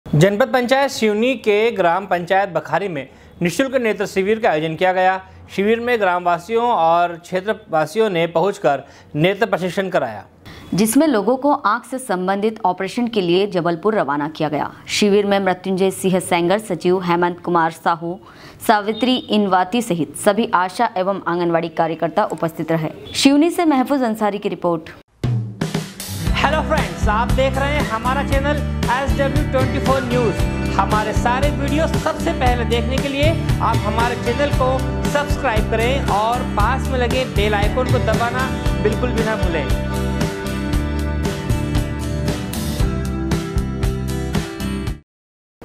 जनपद पंचायत सिवनी की ग्राम पंचायत बखारी में निशुल्क नेत्र शिविर का आयोजन किया गया
जनपद पंचायत सिवनी की ग्राम पंचायत बखारी में निशुल्क नेत्र शिविर का आयोजन किया गया शिविर में ग्राम वासियो ने एवं क्षेत्र वासियों ने पहुंचकर नेत्र परीक्षण कराया